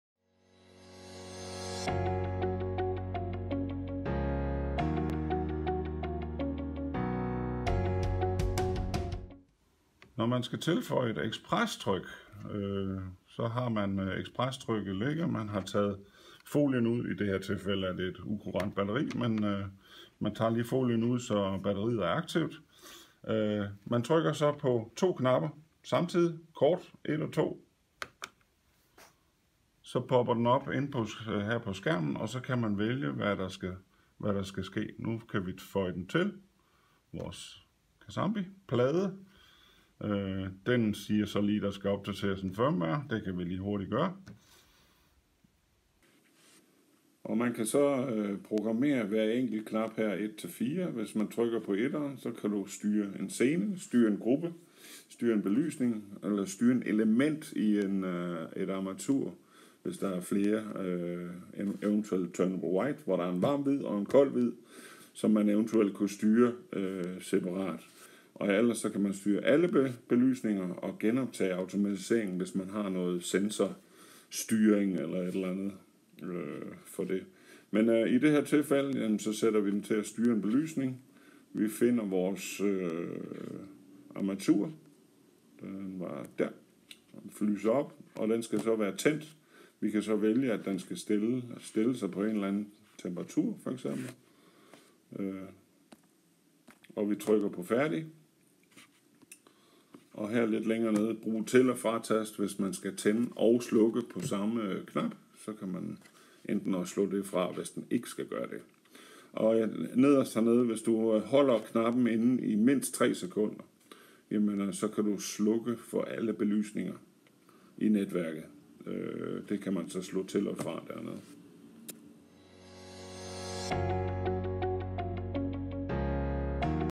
Casambi Ep. 08: Tilføj trådløst Xpress tryk
I denne video viser vi, hvordan man tilføjer et trådløst Xpress tryk i Casambi app'en. Vi gennemgår desuden hvordan man programmerer knapperne på Xpress trykket.